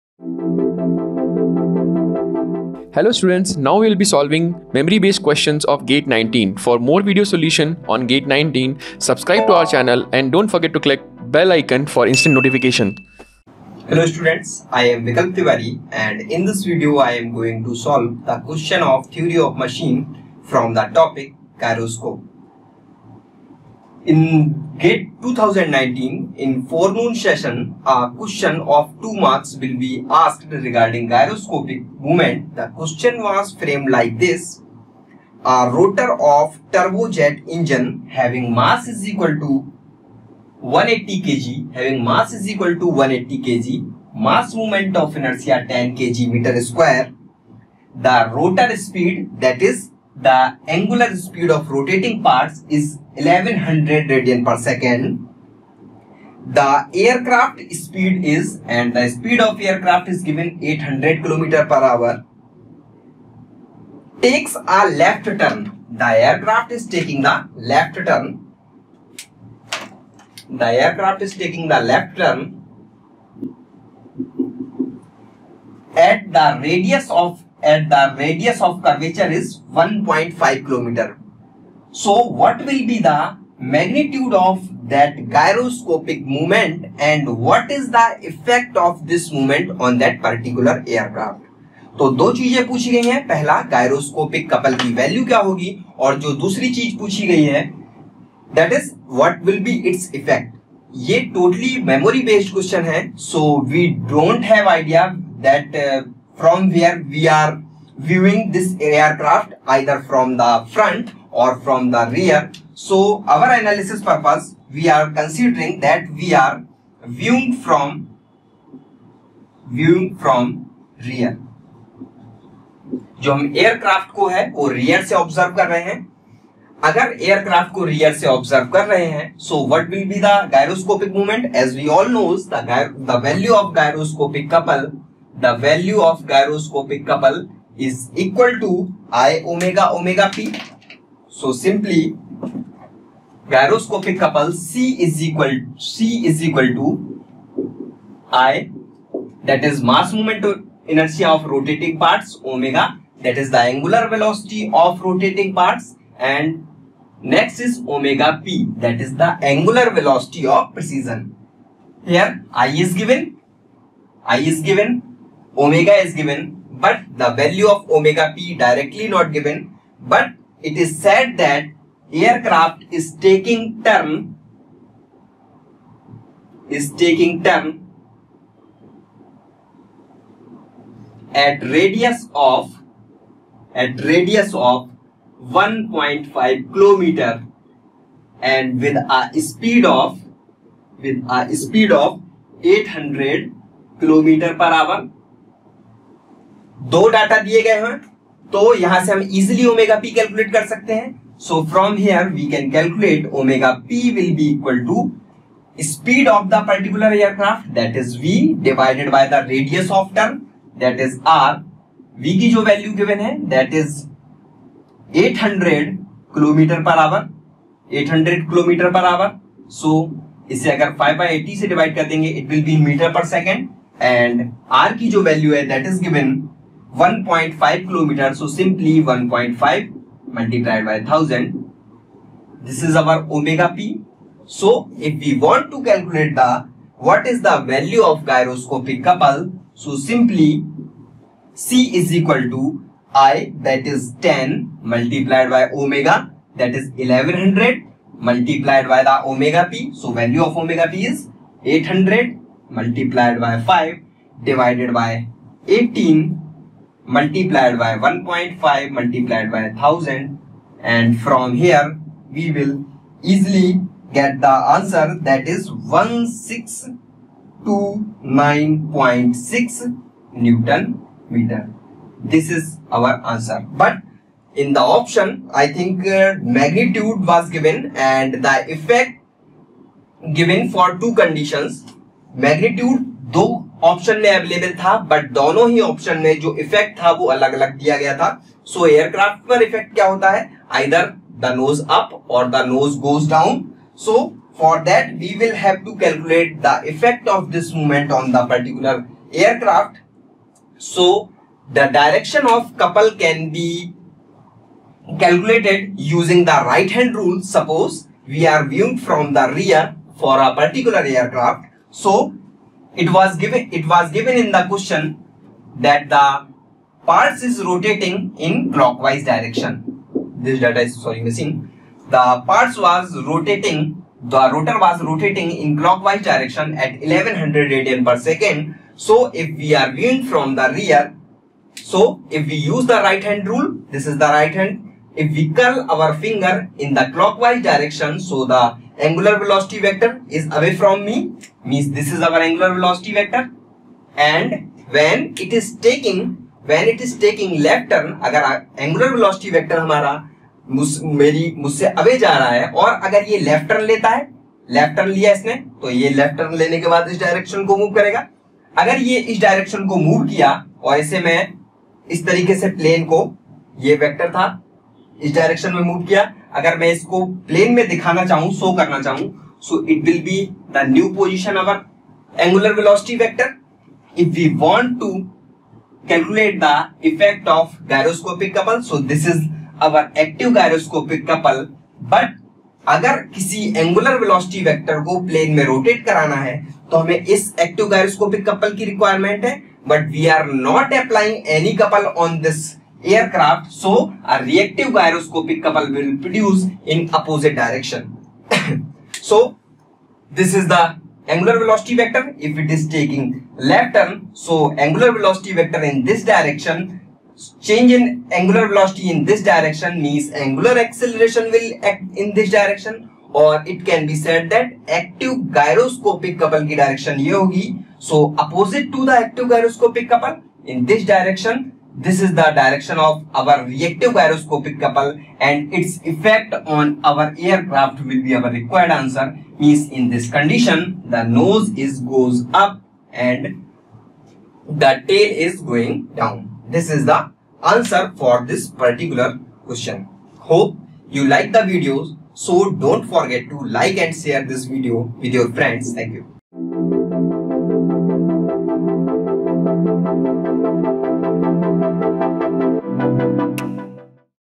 GATE 2019 Answer Key - Paper Analysis for Mechanical Engg (Forenoon) | Theory Of Machines - 03
GATE Paper Analysis Mechanical 2019: GATE exam analysis for GATE Mechanical paper with Answer Key and detailed video solutions.

Do not forget to download GATE 2019 Answer Key for ME with Question Paper and Video Solutions.